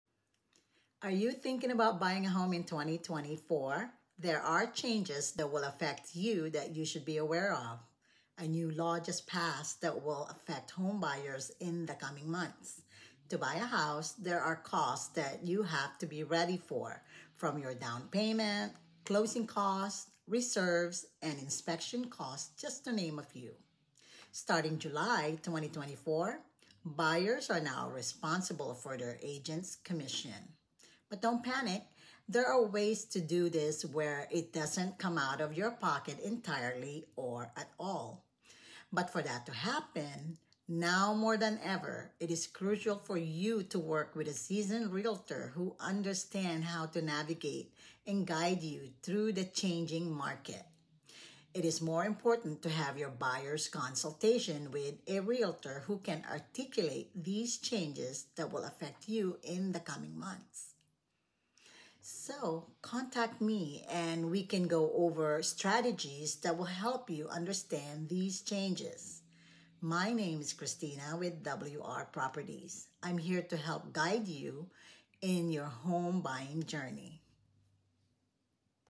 Changes affecting homebuyers in 2024
New law just passed about homebuyers now to pay their agent’s commission.